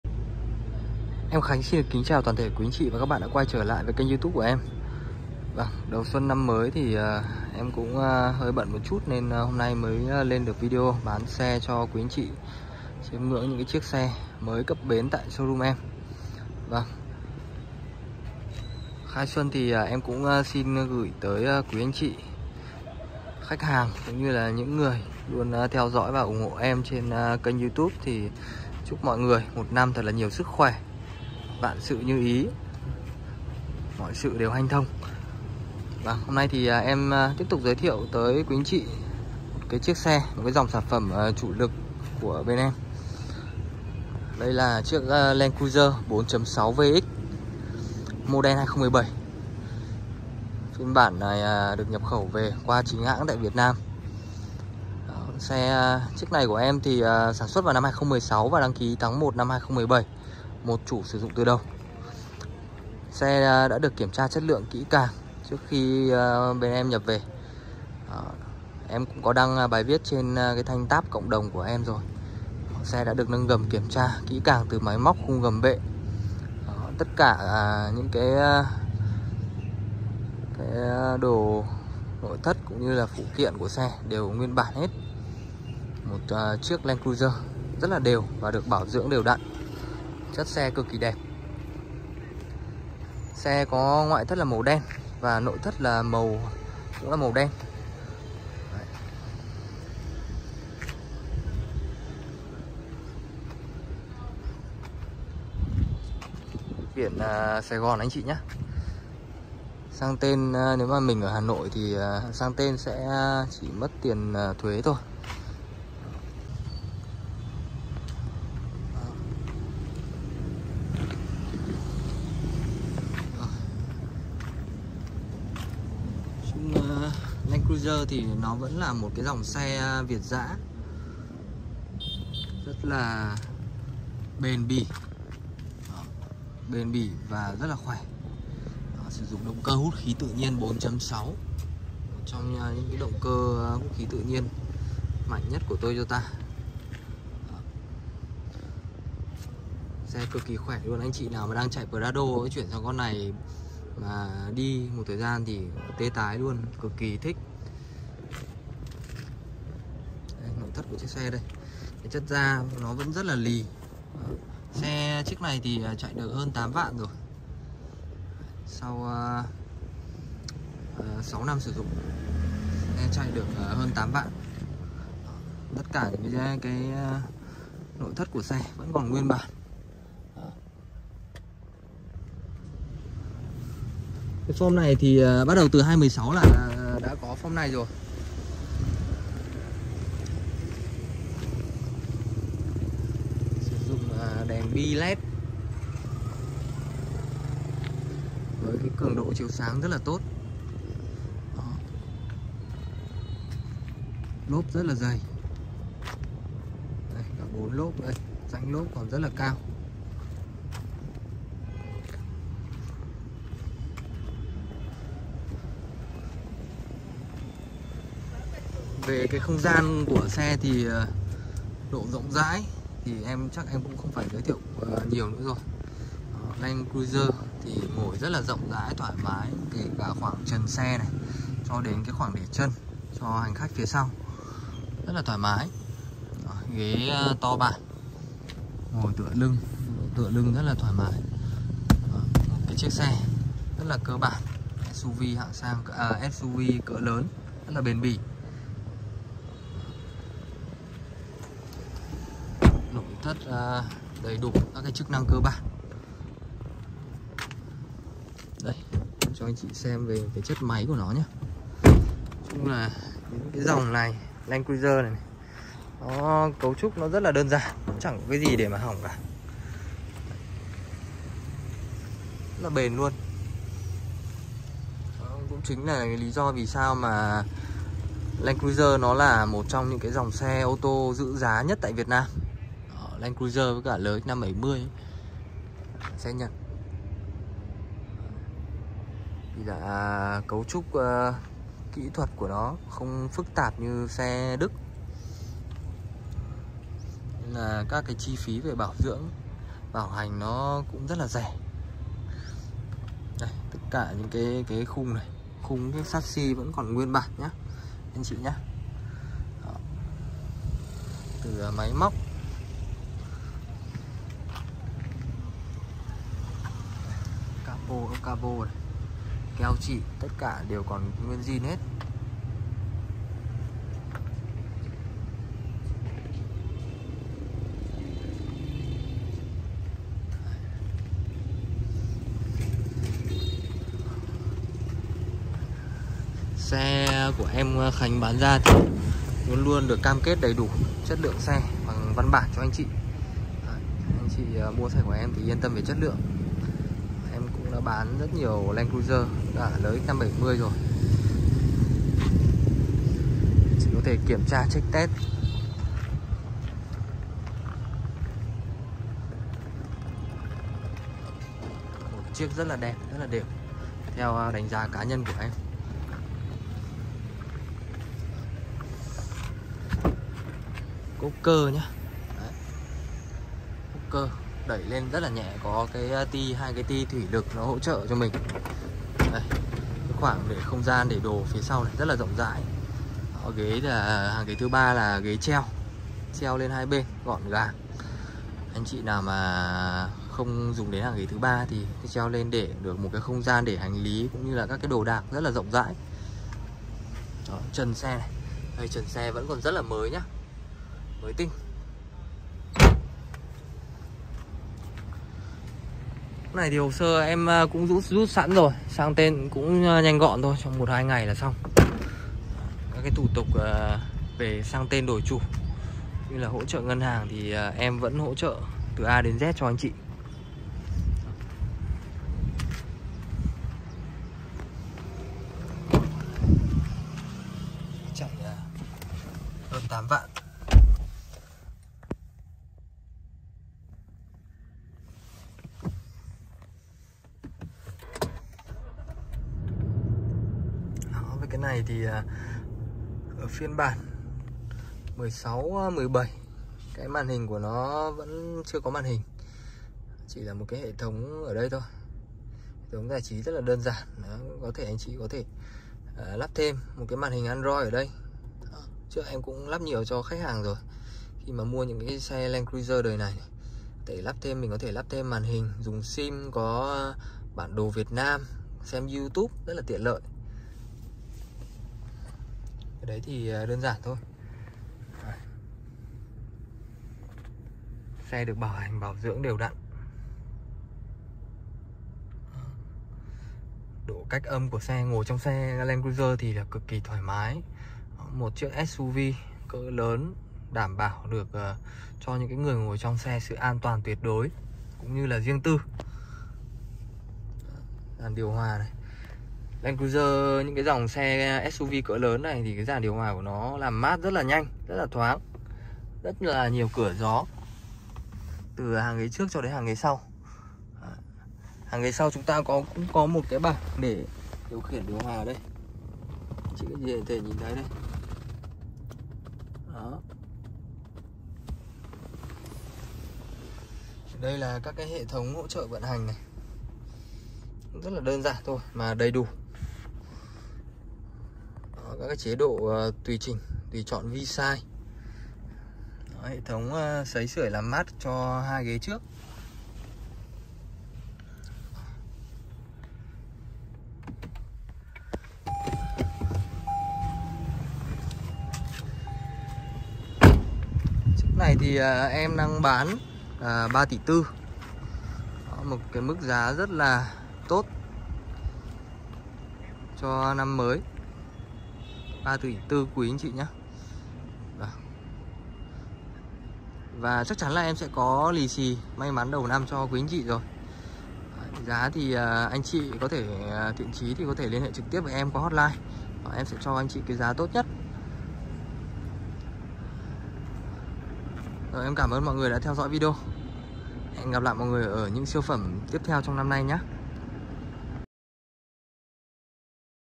Land Cruiser VX model 2017 xe cực chất - 1 chủ từ đầu bảo dưỡng đều đặn, giá tốt đầu xuân !
Duy Khánh - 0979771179 (Zalo, Call, iMess)